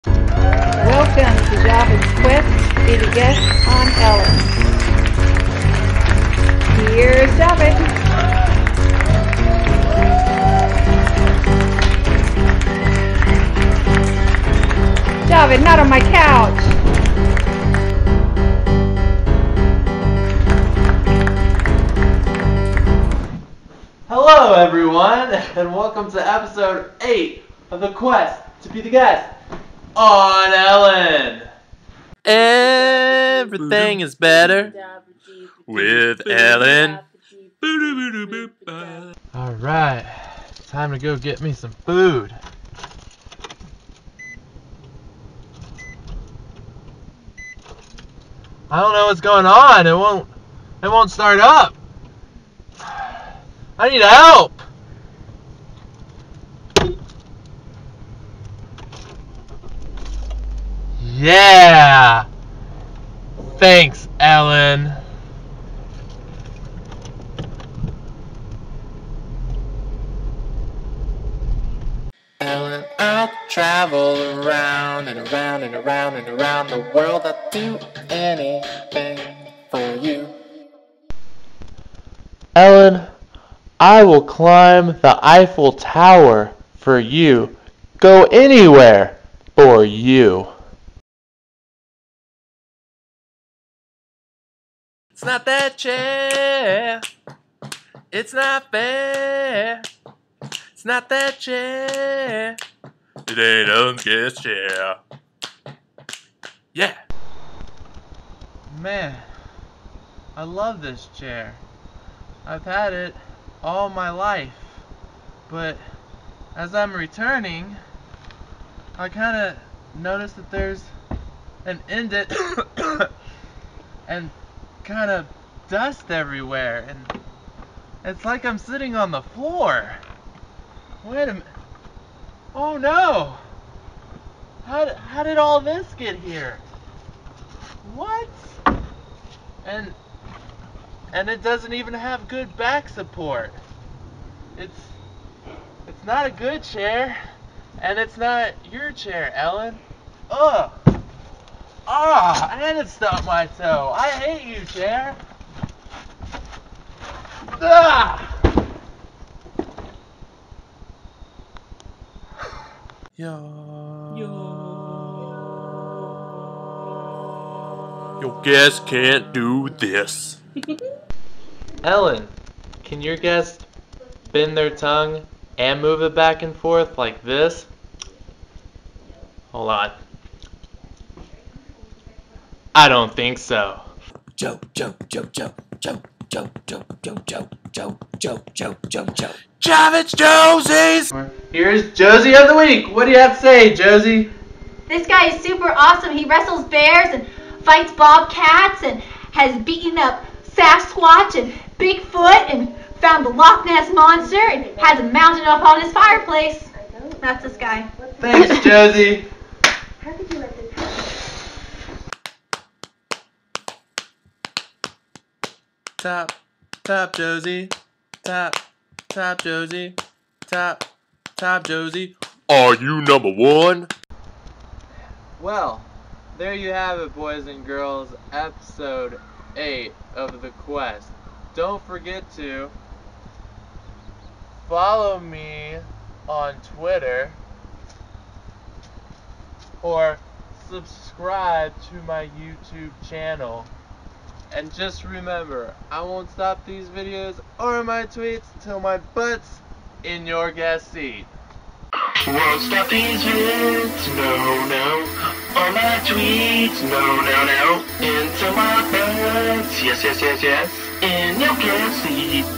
Quest to be the Guest on the Ellen DeGeneres Show! Ep. 8. Murder of a Friend :(
Check out episode 8 where I say goodbye to a real old friend :( Also have an awesome joesie of the week thanks to J.Block!